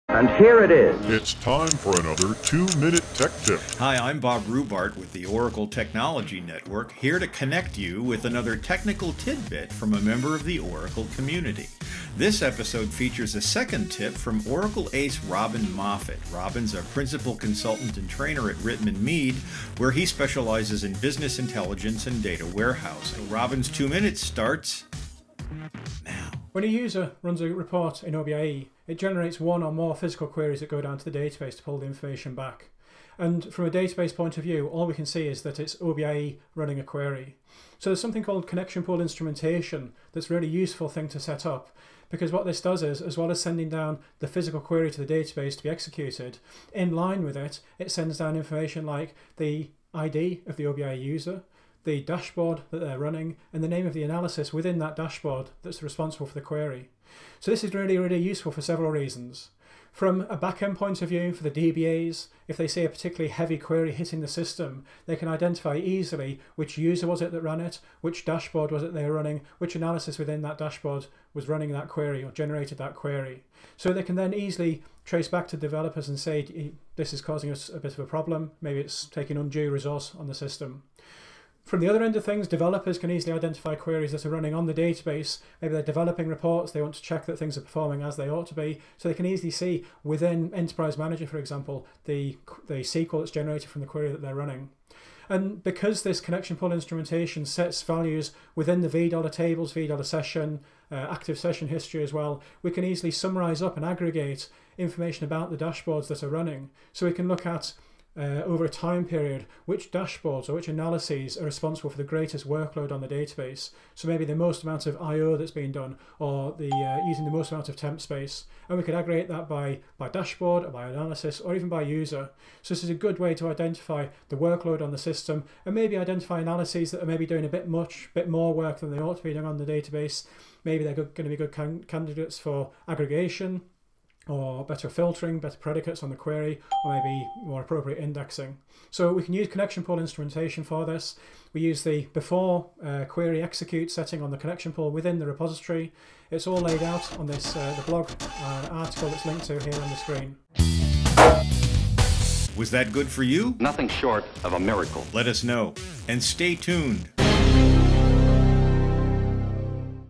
OBIEE and Connection Pool Instrumentation | Robin Moffatt
Oracle ACE Robin Moffatt gives you two minutes and then some with this, his second 2MTT, which dives into Oracle Business Intelligence Enterprise Edition and Connection Pool Instrumentation.

For more information Robin suggests you also read this post on his blog